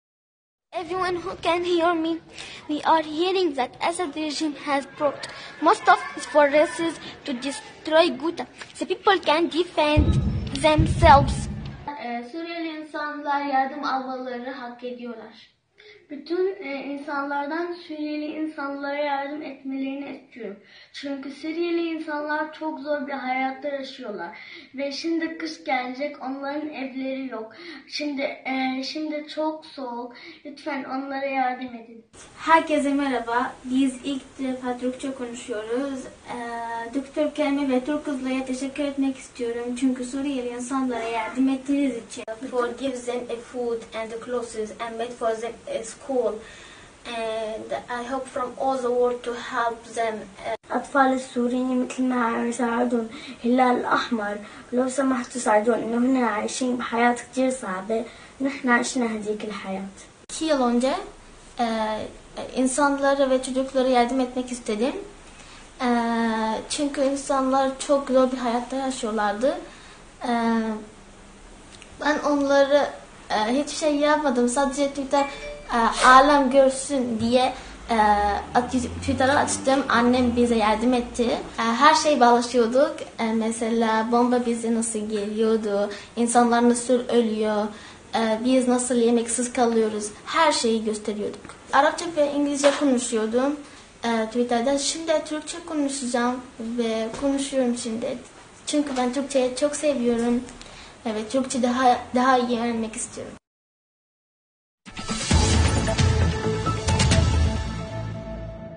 Suriyeli kardeşlerimiz dünyaya üç dilde yardım çağrısında bulunuyor.
"Dünyadaki çocuklara mesajımız. Biz sadece senin gibi yaşamak istiyoruz. Sınırsız yaşamak istiyoruz"